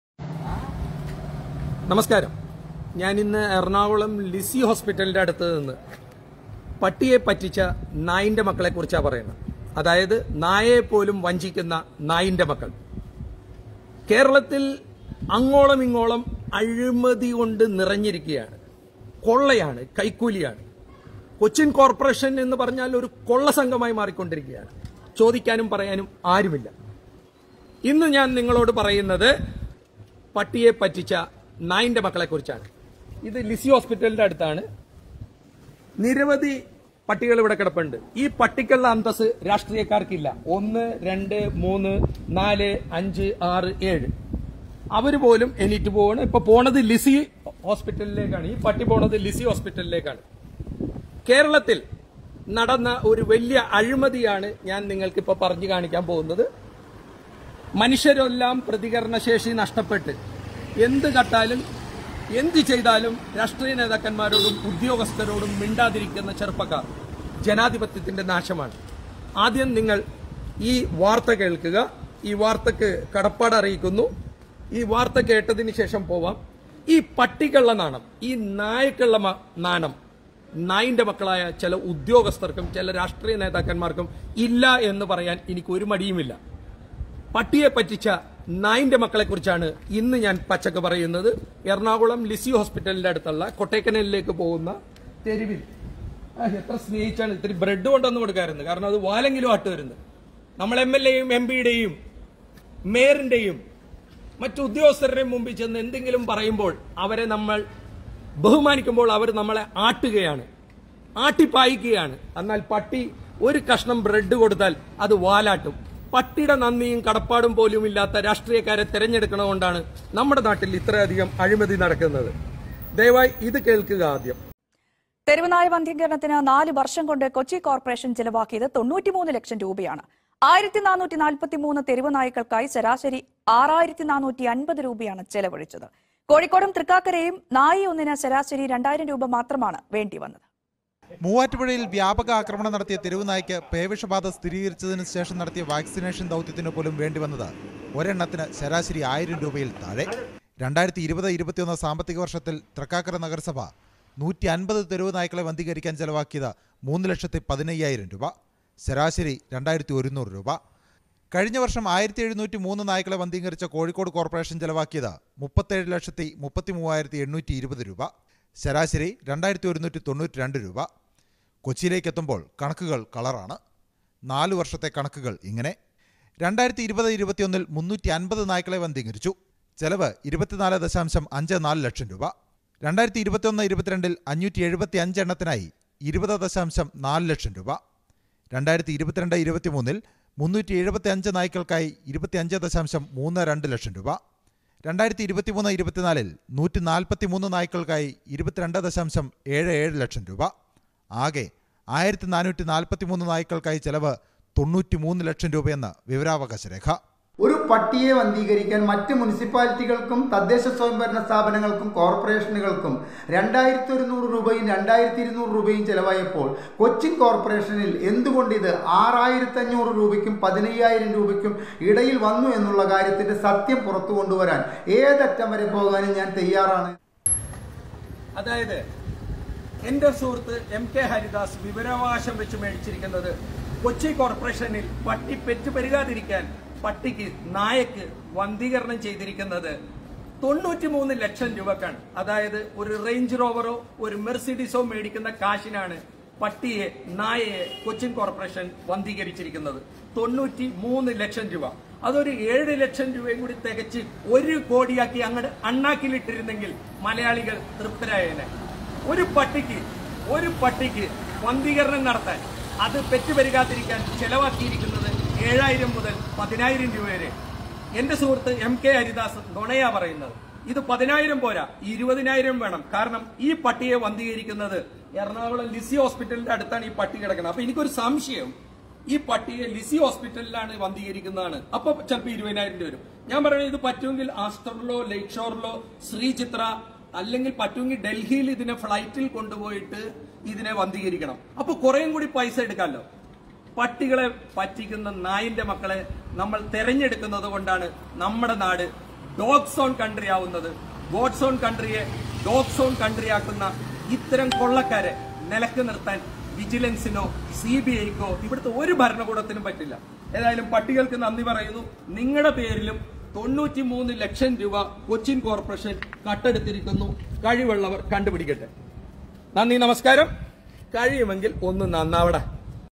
പട്ടിയെ പറ്റിച്ച നായിന്റെ മക്കൾ!
പട്ടിയെ പറ്റിച്ചു 93 ലക്ഷം രൂപ കമ്മി അടിച്ചു മാറ്റി